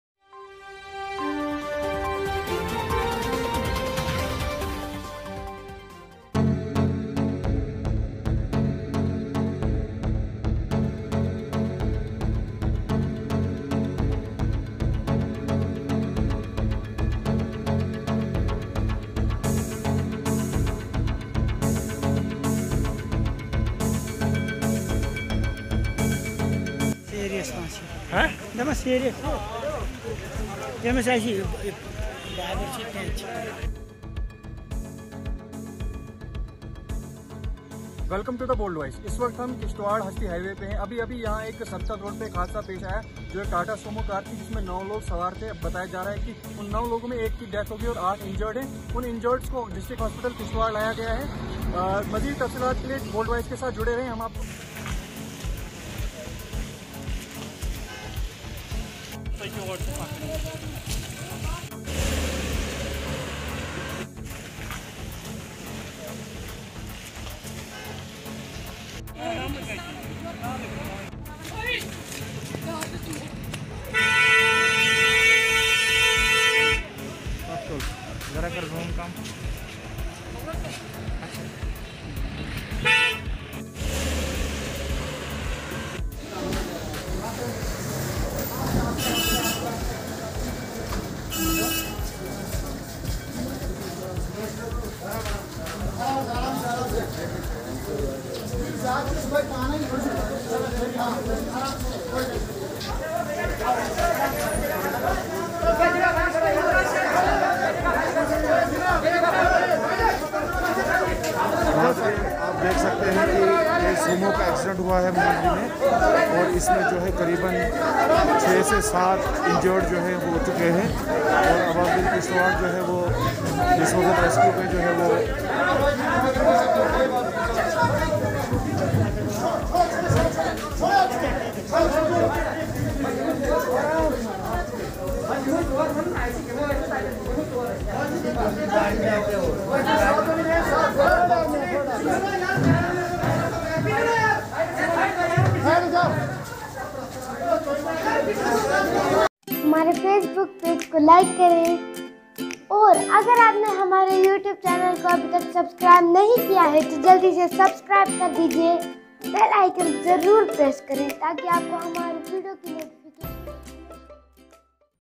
One dead, nine injured in Kishtwar accident.
# Libe Visuals.
One dead, 9 injured in Tata Sumo accident near Hasti on Kishtwar-Hasti Road.